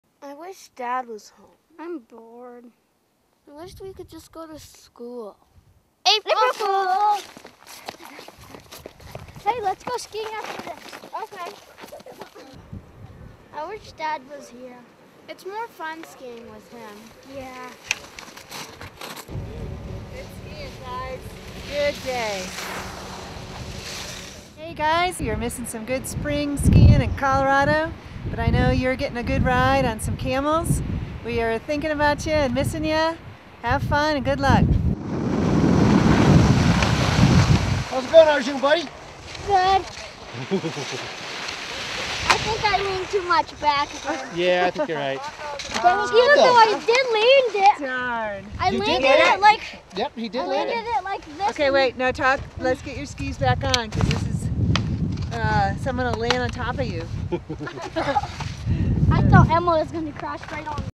Message from home
This is the video that my wife and children made for me, with the help of good friend Rob Raker, that we got to watch in Morocco during Expedition Impossible.